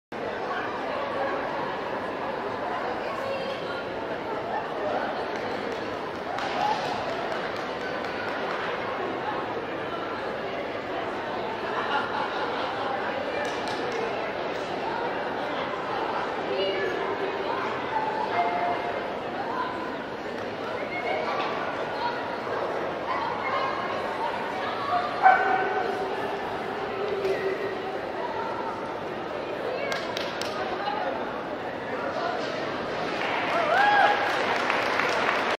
NovaSat_Excellent_JWW_Rose City Classic AKC_January_2013
Nova, a Belgian Tervuren, runs AKC Masters Agility at Rosecity Classic 2013